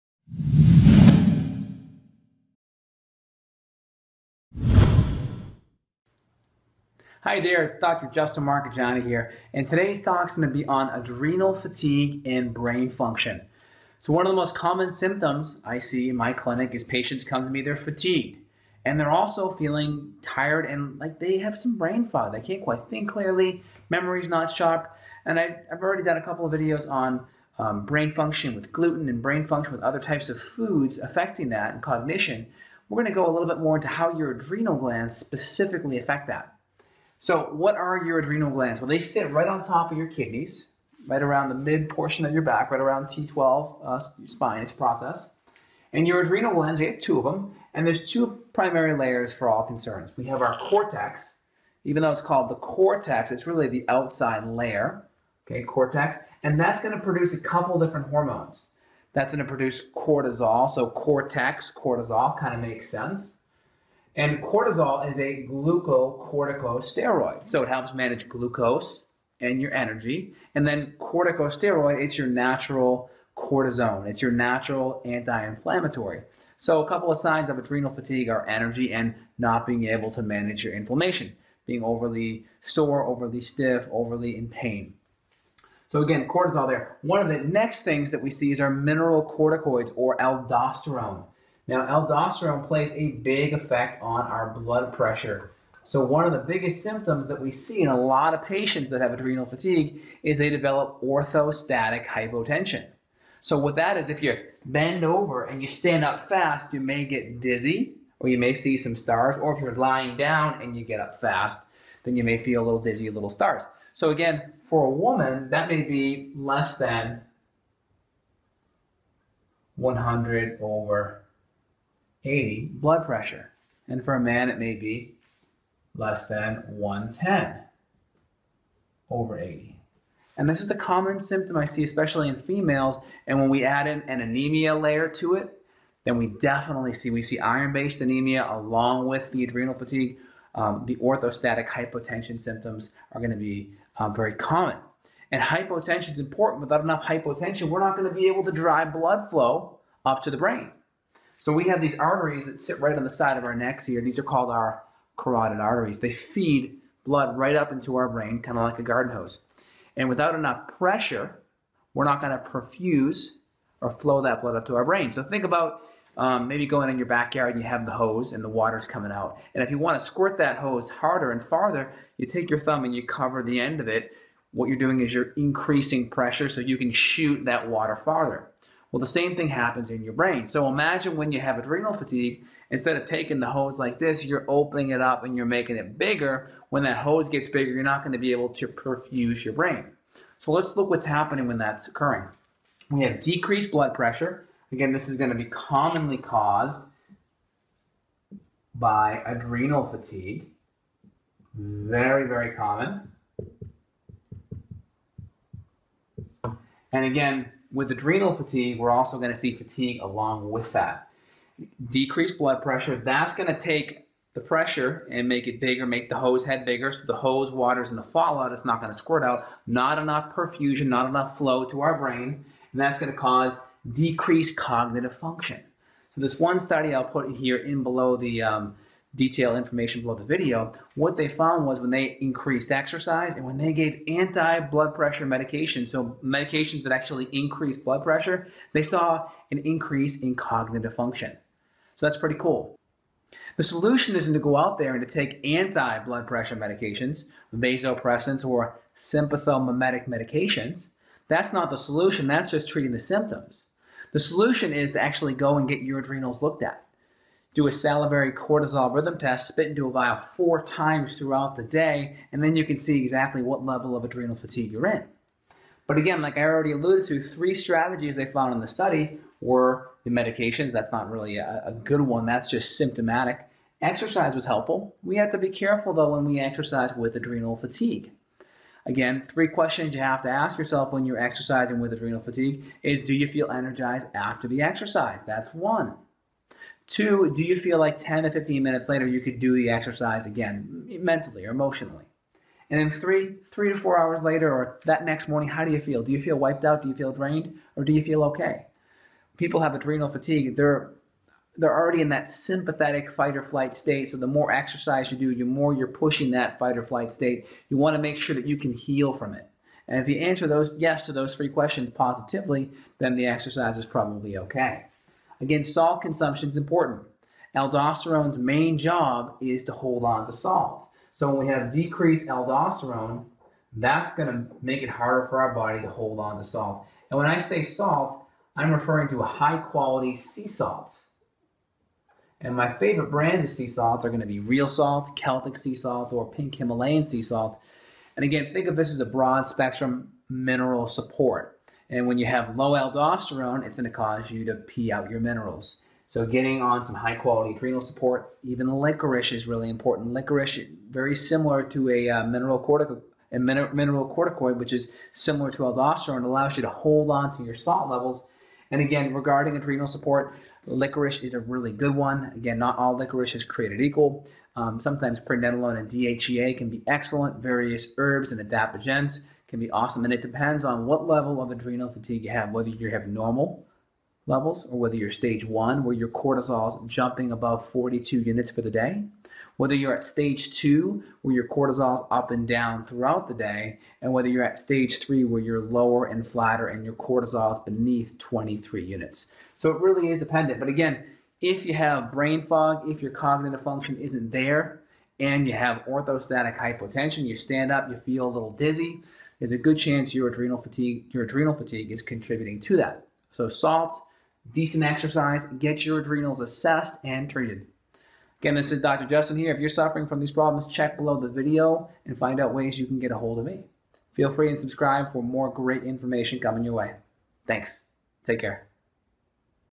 Adrenal Fatigue - How It Effects Brain Function and Low Blood Pressure
Adrenal fatigue - Brain function and low blood pressure

In this video, Dr. Justin Marchegiani talks about the effects of adrenal fatigue to your brain and body functions. Do you feel like your adrenal fatigue is preventing your brain from functioning optimally? One of the biggest symptoms of adrenal fatigue is low blood pressure. When your blood pressure is low it's more difficult to deliver blood to your brain. With adrenal fatigue, you won't be able to have optimal brain function and you may even have brain fog because of adrenal fatigue. Watch this video to learn more about the effects of adrenal fatigue and the things you can do to help prevent it.